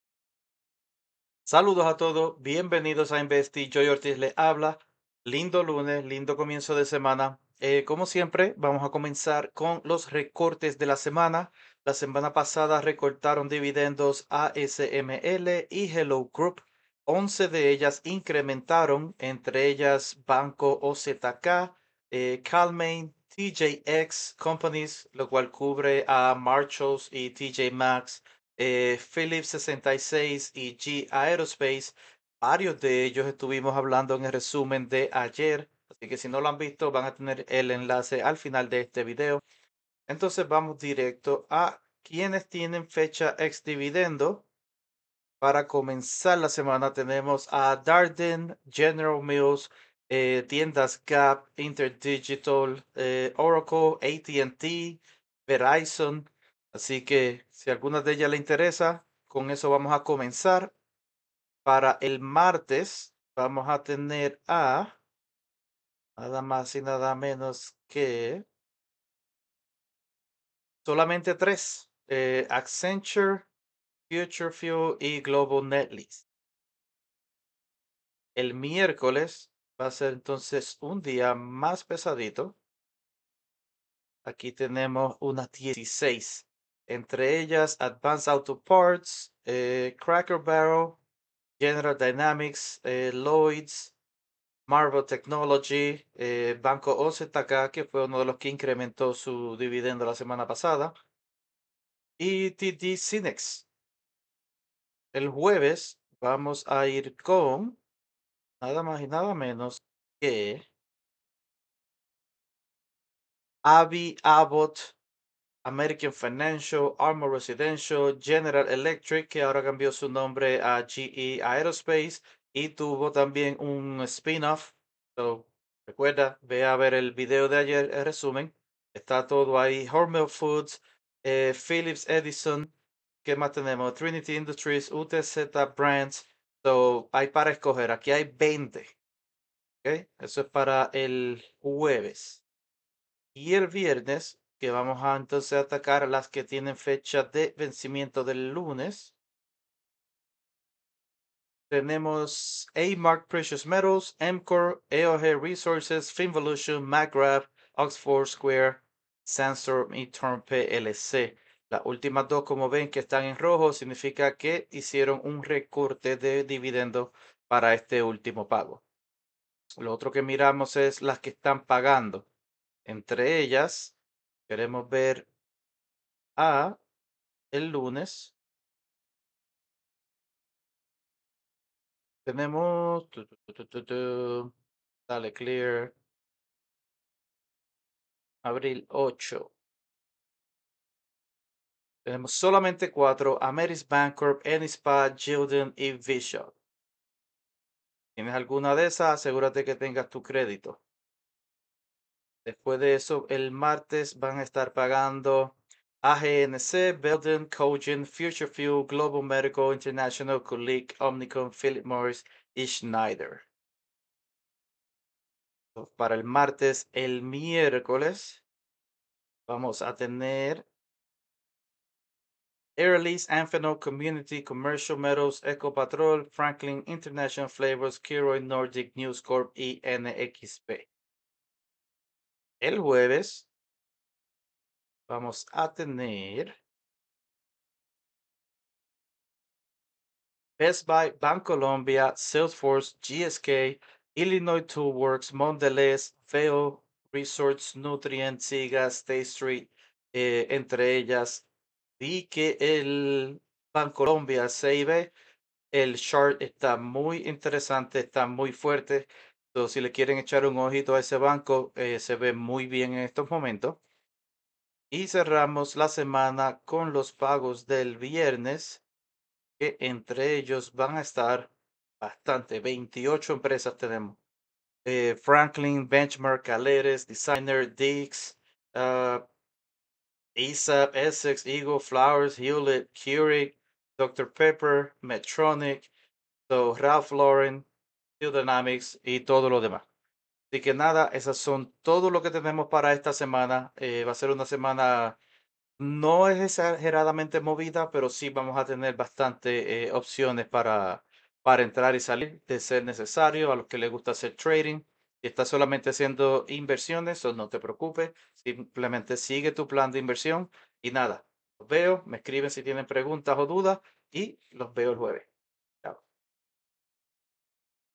dividendos Semana 8-12 Abril
Todos los recortes, aumentos, fechas ex-dividendos y pagos para la semana del 8-12 Abril, 2024.

Si tienes preguntas, no dudes en escribirme.